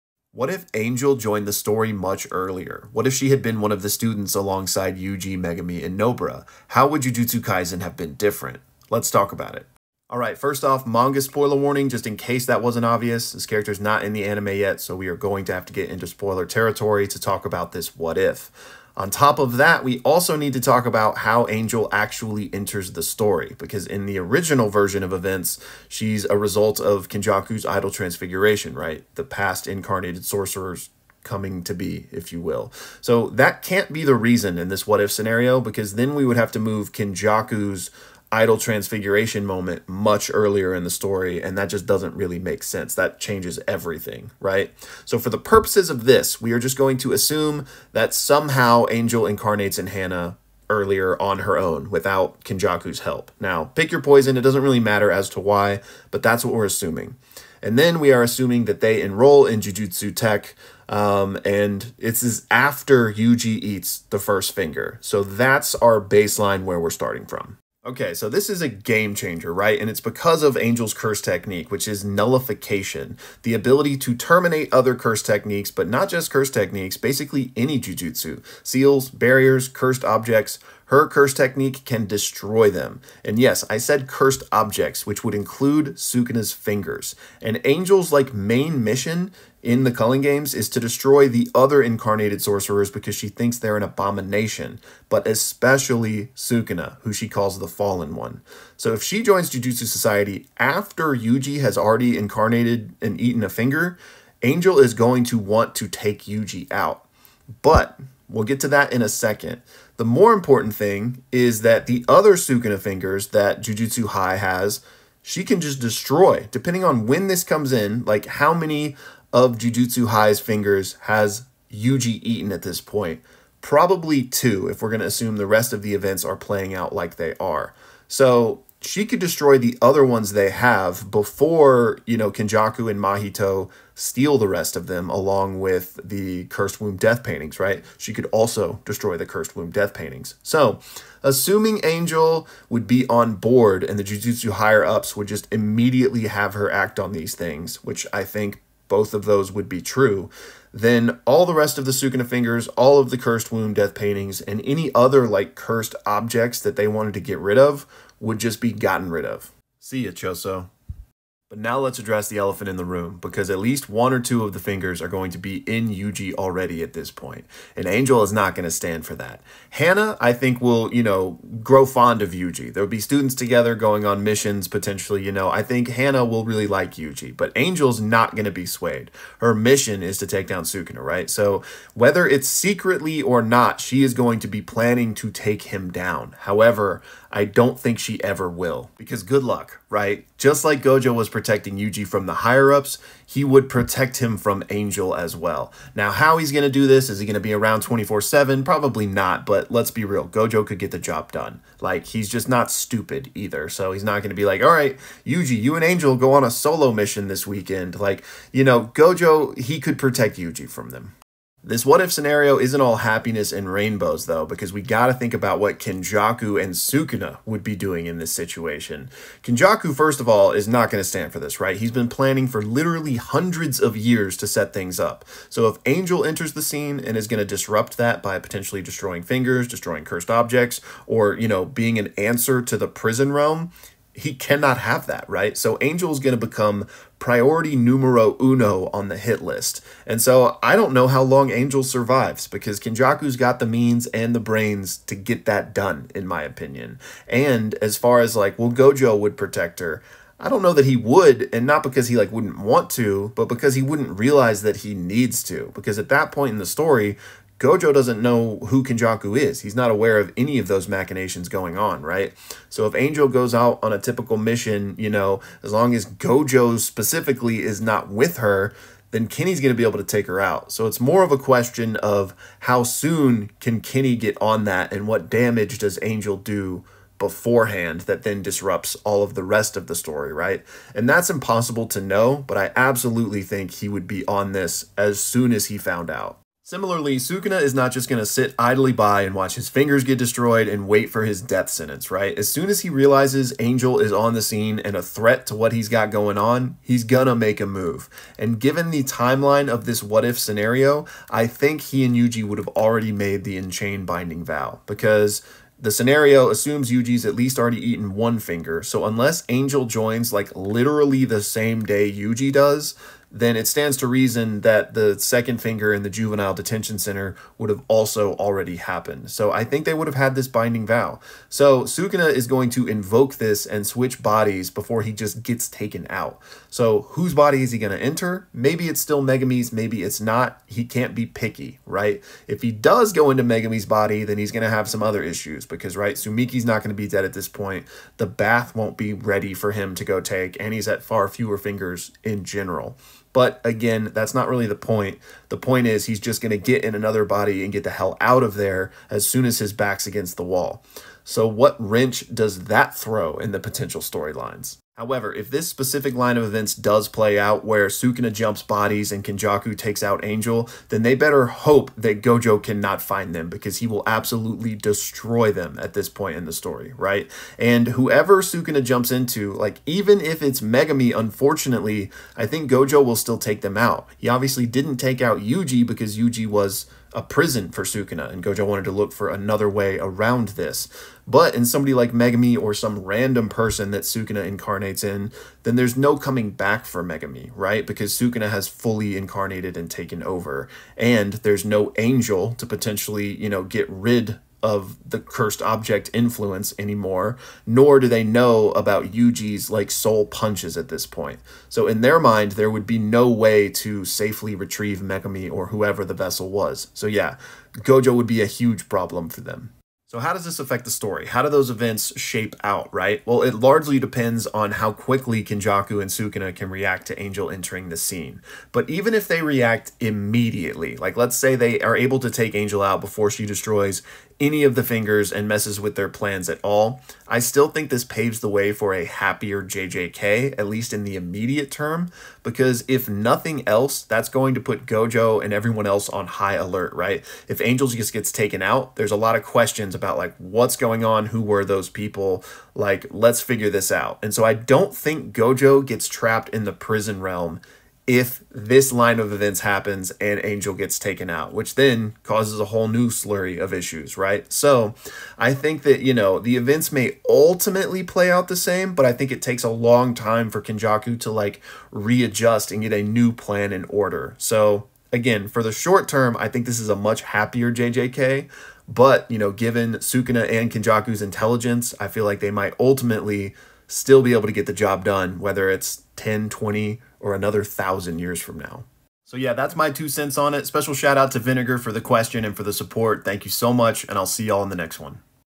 What If Angel Went to Jujutsu Tech? | Jujutsu Kaisen
What if Angel had been introduced in the story far earlier, what would have happened?